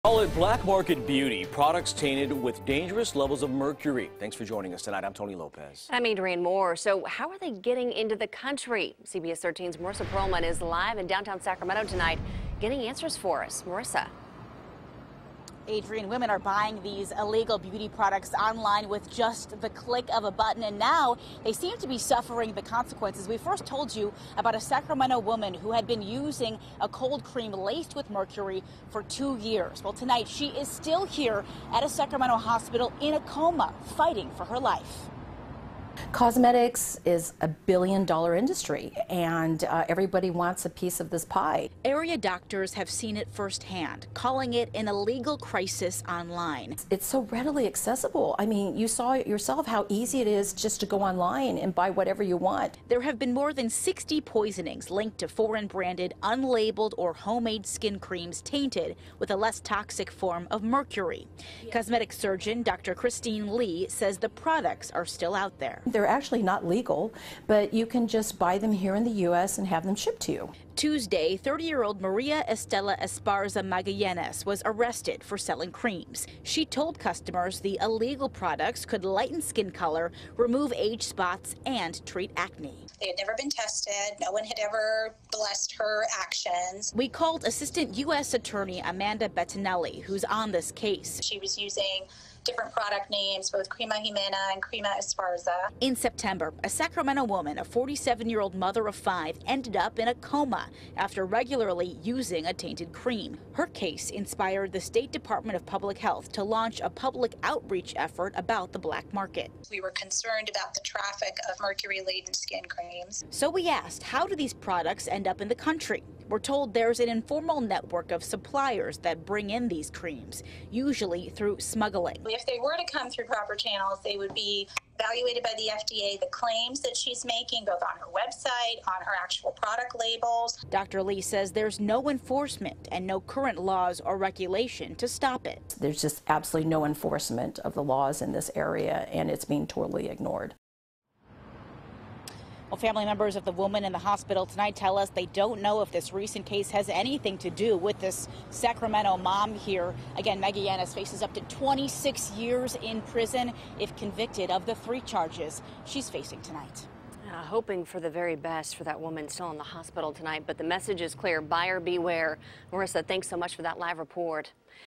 Tainted Skin Cream Warning
Women are buying illegal beauty products with the click of a button and are now suffering the consequences.